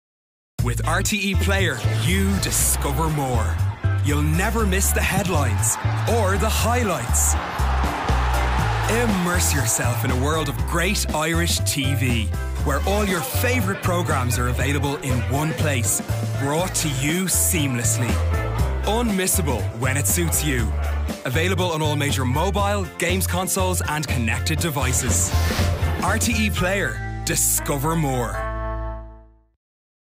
RTÉ Player 'Discover More' 30"
30 Second TV Commercial for the Player, devised and produced by RTÉ Commercial Studios to promote the 'seamless' availibility of great Irish TV on all devices!